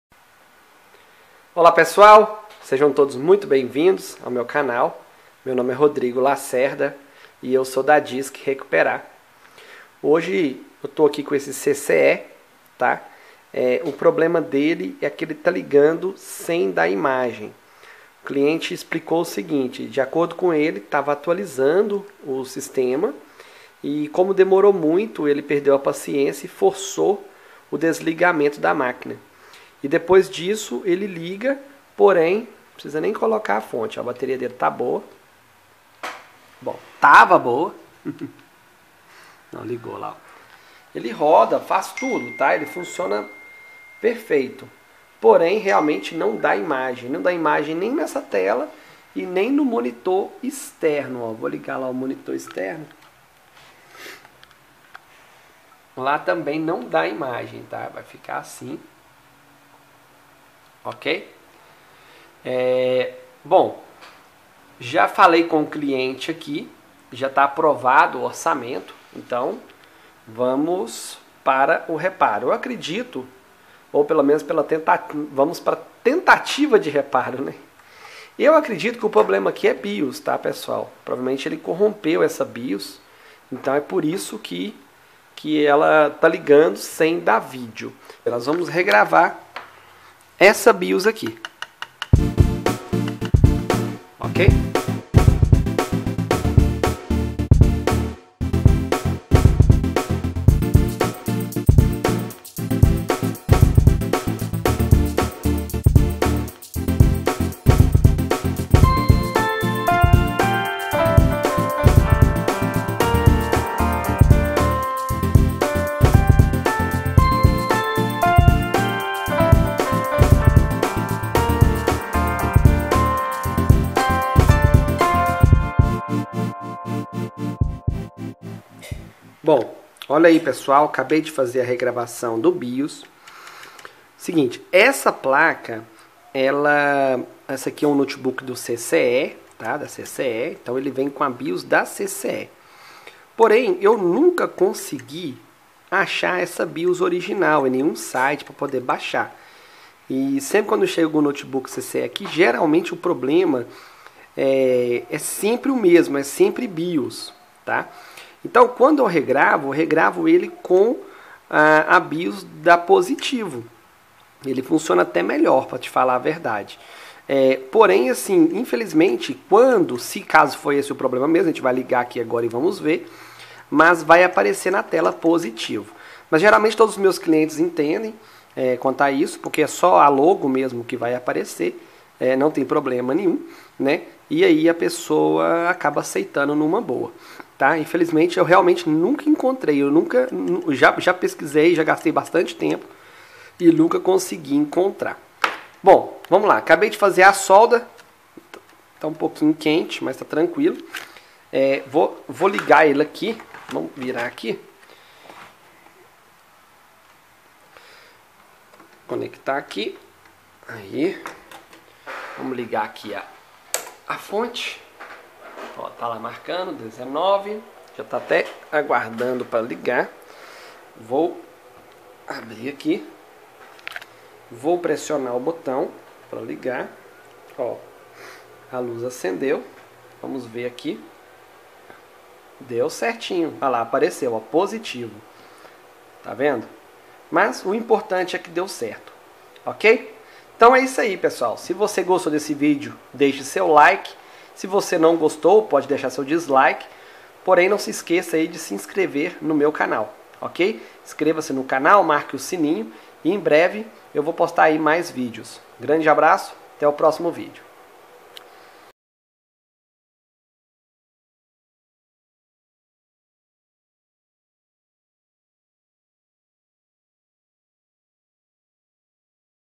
CCE que virou POSITIVO
S E  I N S C R E V A  N O  C A N A L

Obrigado a todos que estão me ajudando a chegar a minha meta de inscritos!!!

Neste vídeo reparo um notebook cce que não gera imagem. Assista e descubra o que era.